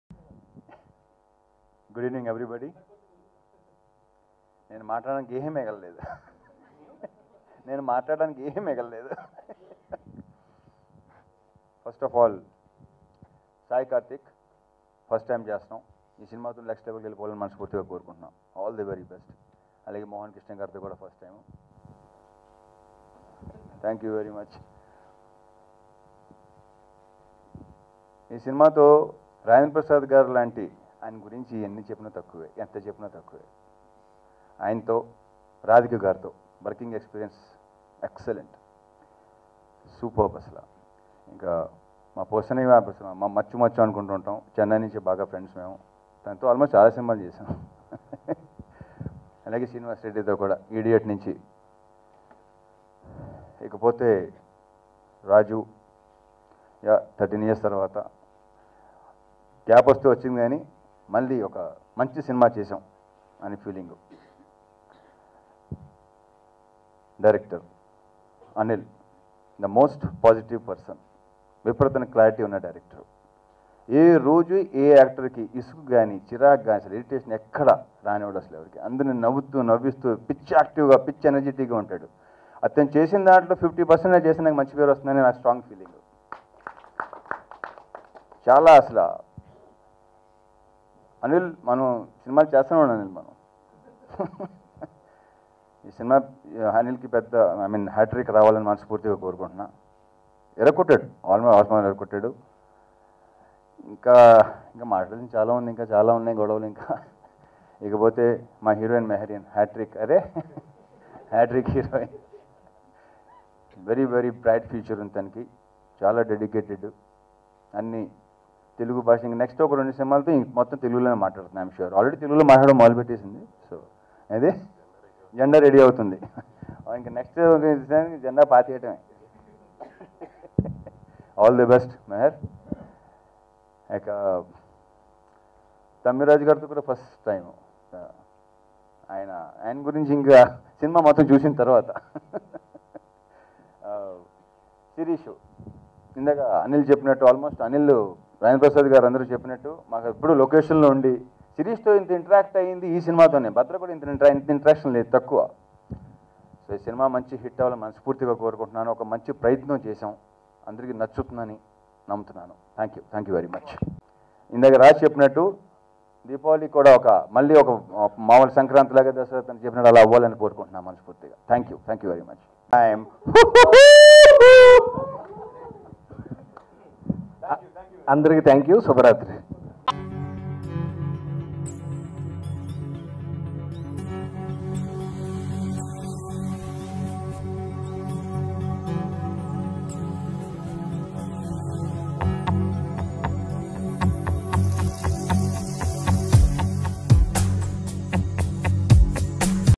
Ravi Teja Superb Funny Speech || Raja The Great Movie Trailer Launch || Mehreen || Anil Ravipudi
Watch Ravi Teja Superb Funny Speech at Raja The Great Movie Trailer Launch . Raja The Great latest 2017 Telugu movie,Ravi Teja,Mehrene Kaur. Directed by Anil Ravipudi. Produced by Dil Raju under the banner Sri Venkateswara Creations. Music by Sai Kartheek.